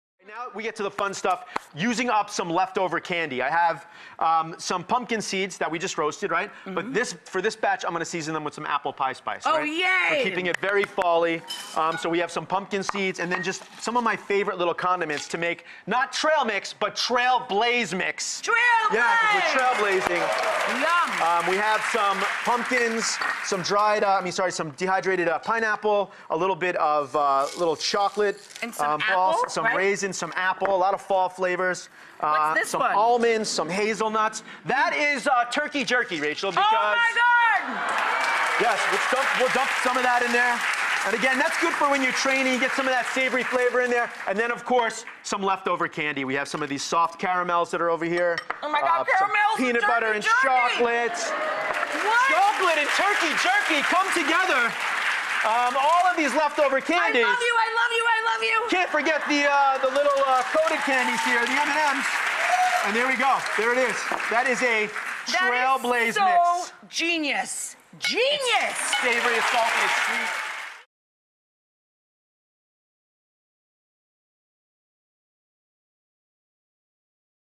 Richard Blais’ Pumpkin Seed Trail Mix
This is the perfect use up for Halloween leftovers.